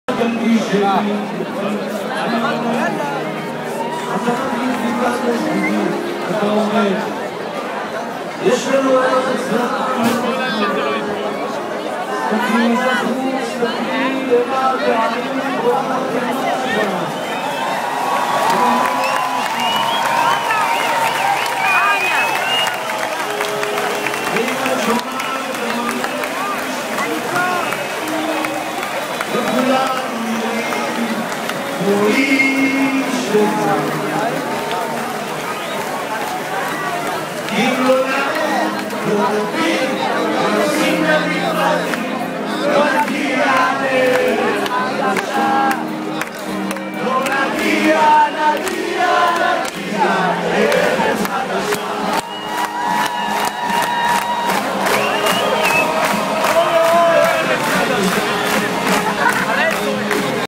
העם החליט צדק חברתי - ממש צפוף, אנשים על בניין
הפגנת הענק בתל-אביב בתאריך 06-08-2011 - תחת הכותרת "העם החליט צדק חברתי"